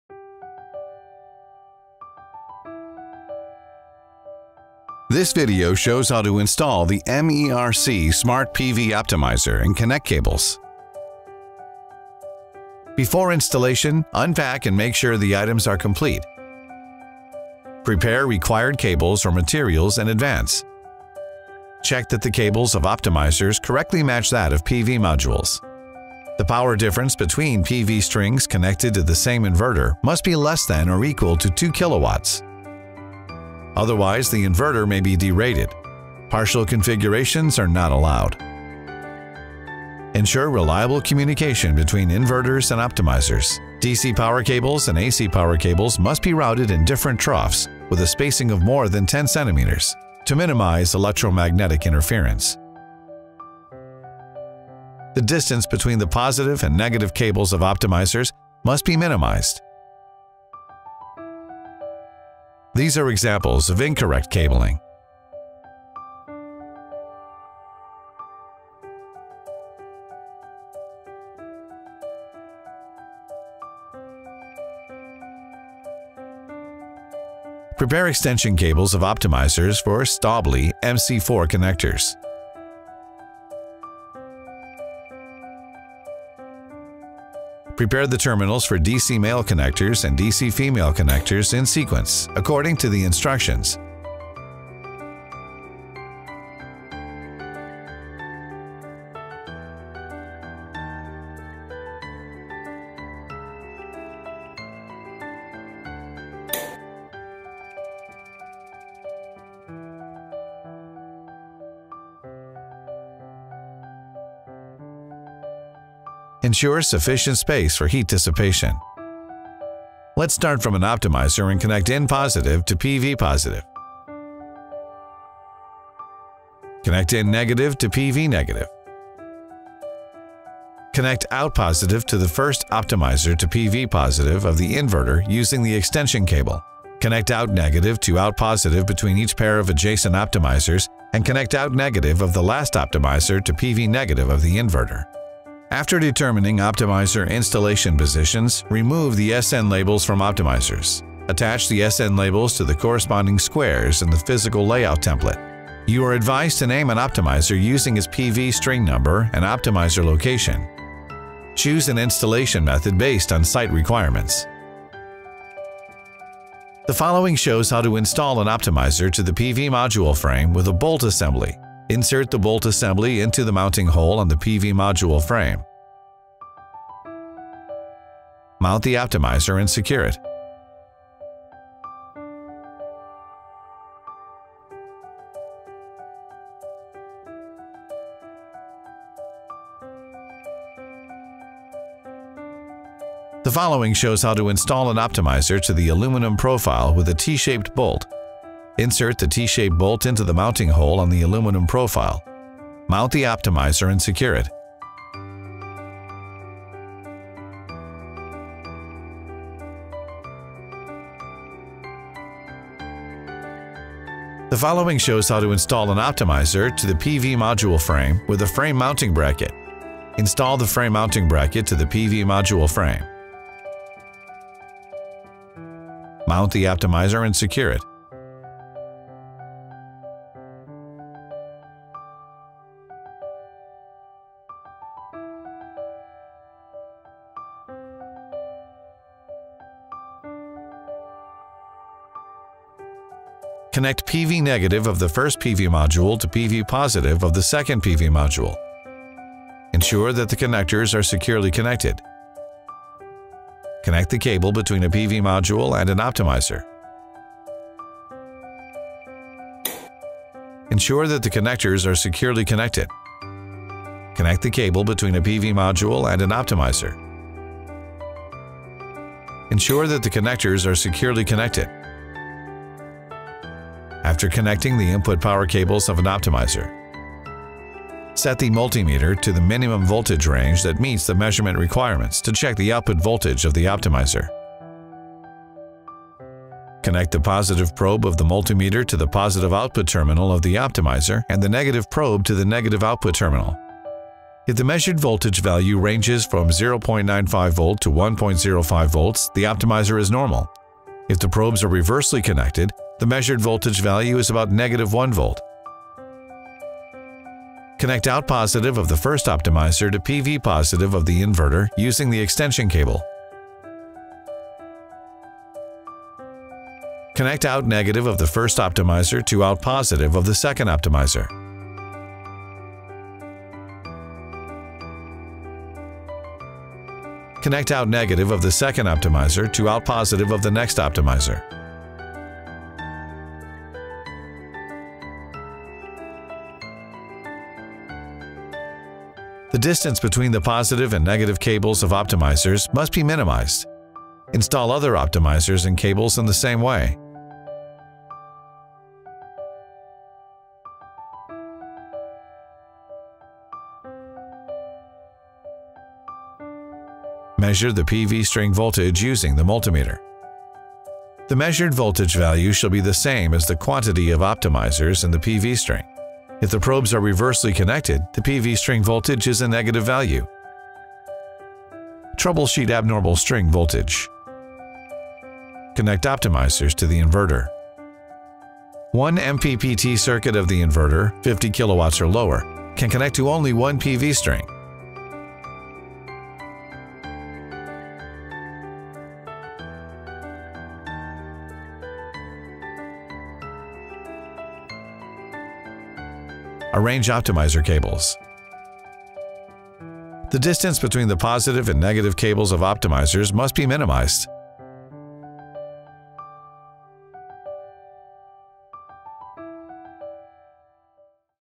Learn the correct way to install the Smart PV Optimiser
1. Make sure that the string configuration is reasonable, and know the requirements of the optimiser "accompanied by the alignment".
2. Make extension cables to ensure proper wiring.
3. Insert the physical layout diagram, according to the actual situation, choose a mounting method to install.
4. Optimisers are wired "hand in hand", measure the voltage between positive and negative strings to ensure the optimisers are installed correctly.